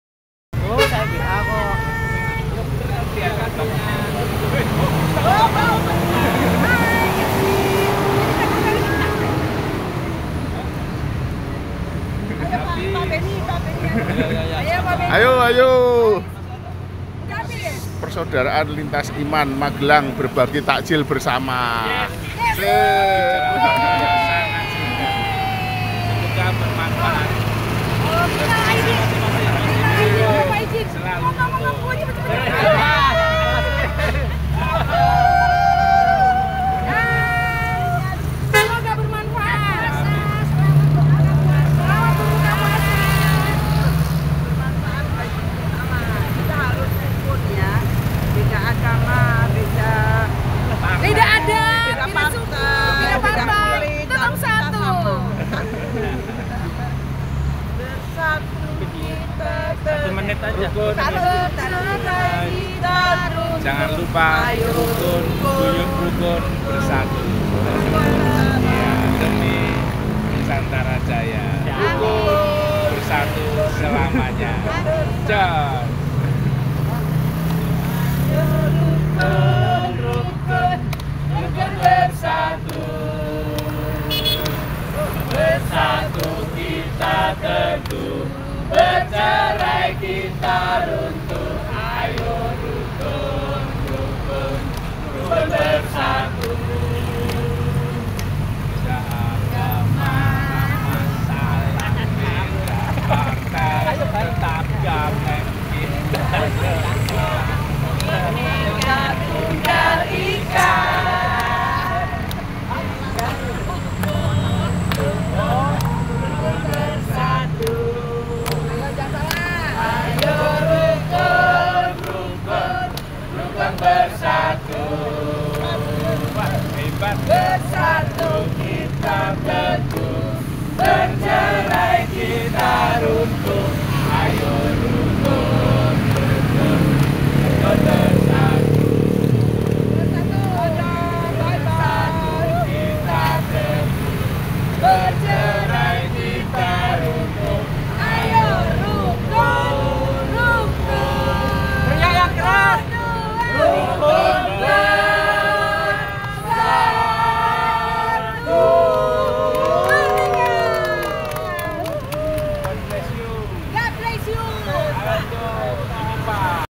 PERSAUDARAAN LINTAS IMAN MAGELANG | BERBAGI TAKJIL GRATIS
Lokasi Terminal Bus Tidar Kota Magelang

Keramaian menjelang Buka Puasa Ramadhan 2024

Magelang street view, motovlog Indonesia